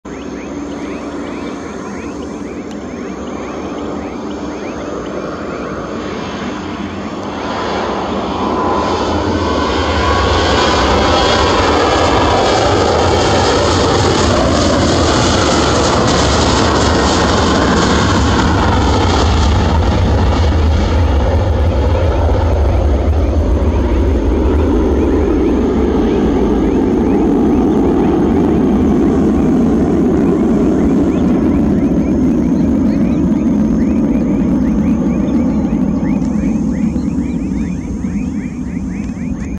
DECOLAGEM DO Boeing 737-8EH DA GOL PR-GUM AEROPORTO DE TERESINA
PR-GUM  Boeing 737-8EH - GOL Linhas aéreas SBTE / THE
TERESINA, BRAZIL